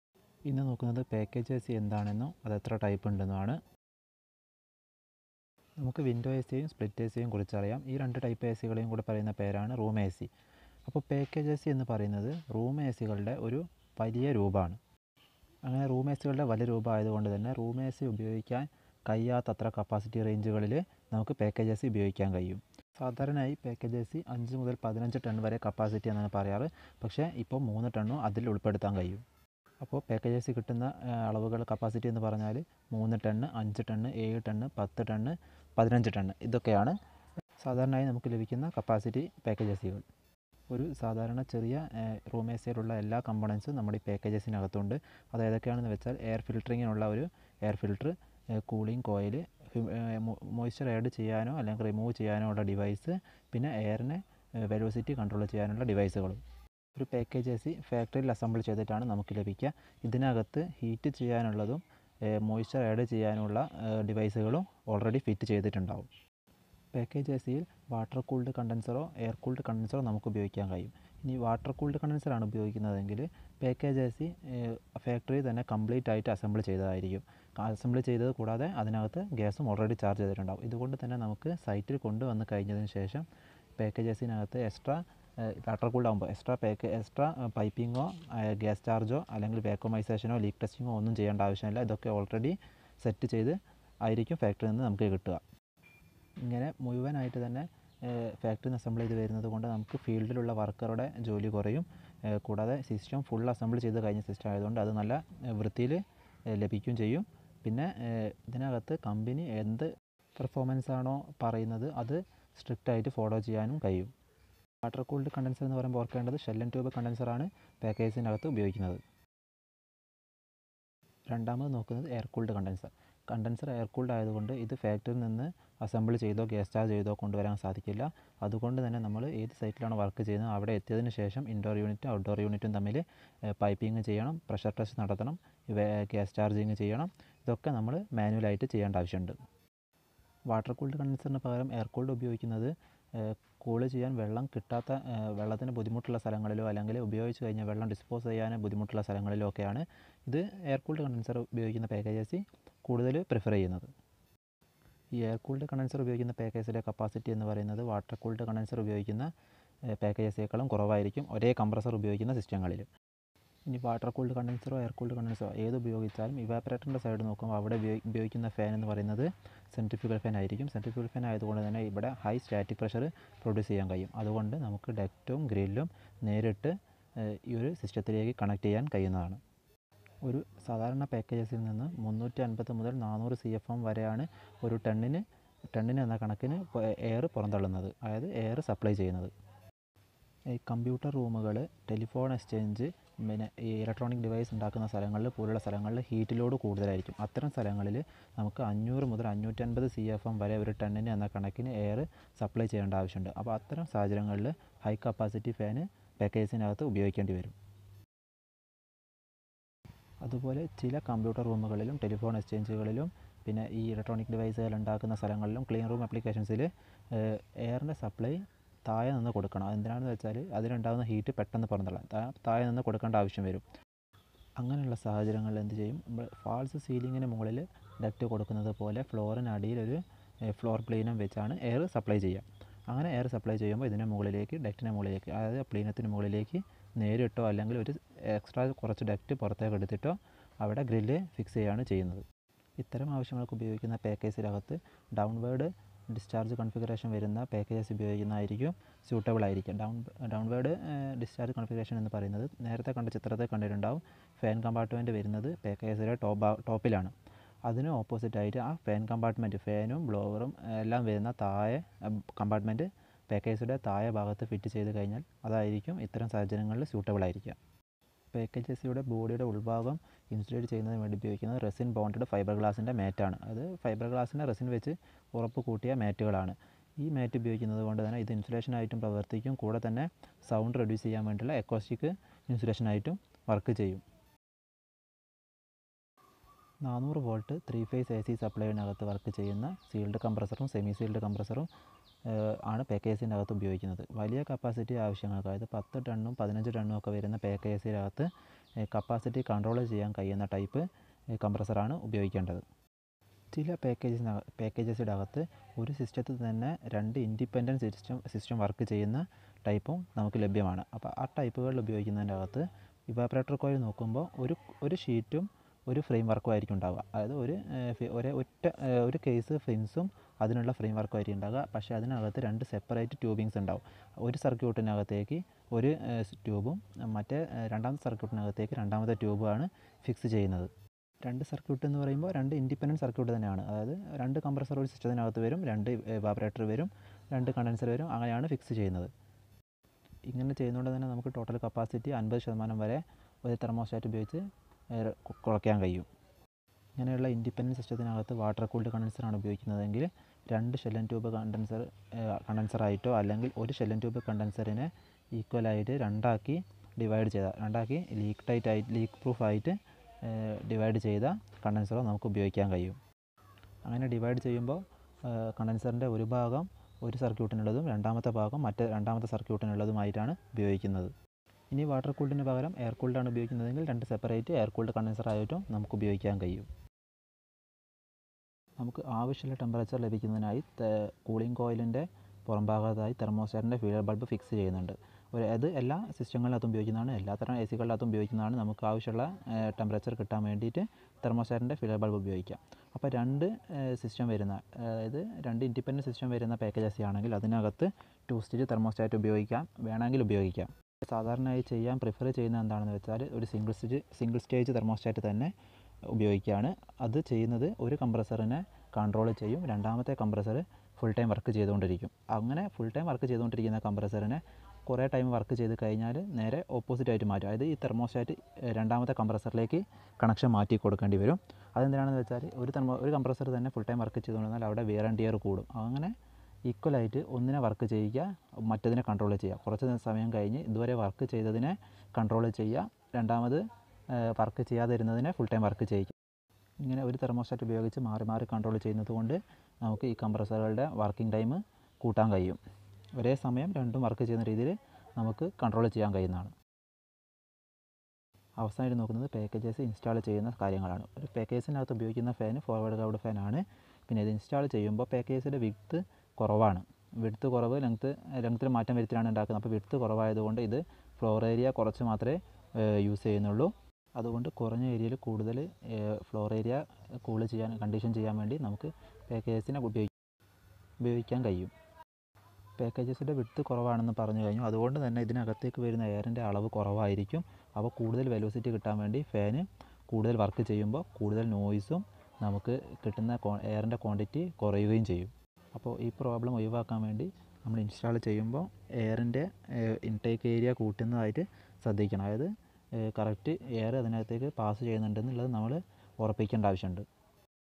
Package air conditioner Malayalam
Theory lesson about package ac for iti MRAC trainees in malayalam